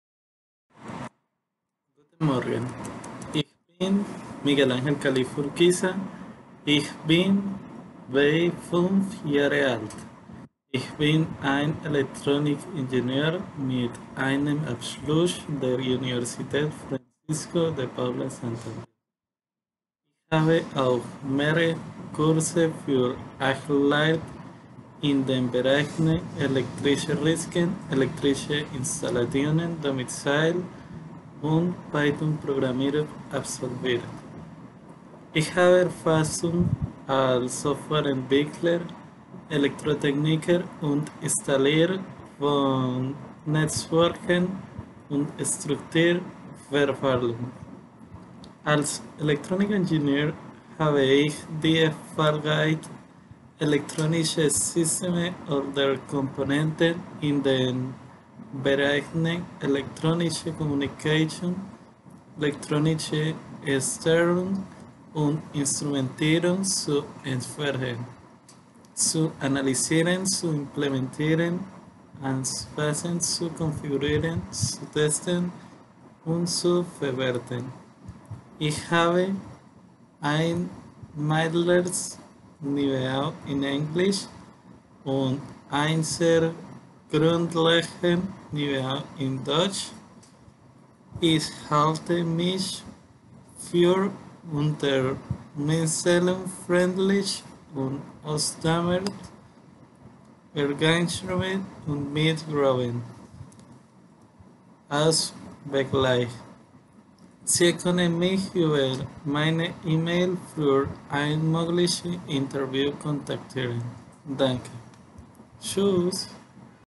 DE_CV Miguel Califa
Transkription.

Guten Morgen.

Ich bin Miguel Angel Califa Urquiza, ich bin 25 Jahre alt. Ich bin ein Elektronikingenieur mit einem Abschluss der Universität Francisco de Paula Santander. Ich habe auch mehrere Kurse für Sicherheit in den Bereichen elektrische Risiken, elektrische Installationen, Domizile und Python-Programmierung absolviert.

Ich habe Erfahrung als Software-Entwickler, Elektrotechniker und Installateur von Netzwerken und strukturierter Verkabelung.

Als Elektronikingenieur habe ich die Fähigkeit, elektronische Systeme oder Komponenten in den Bereichen elektronische Kommunikation, elektronische Steuerung und Instrumentierung zu entwerfen, zu analysieren, zu implementieren, anzupassen, zu konfigurieren, zu testen und zu bewerten.

Ich habe ein mittleres Niveau in Englisch und ein sehr grundlegendes Niveau in Deutsch, ich halte mich für unternehmungslustig, freundlich und ausdauernd, ergebnisorientiert und mit einer großen Anpassungsfähigkeit.

Sie können mich über meine E-Mail für ein mögliches Interview kontaktieren, danke.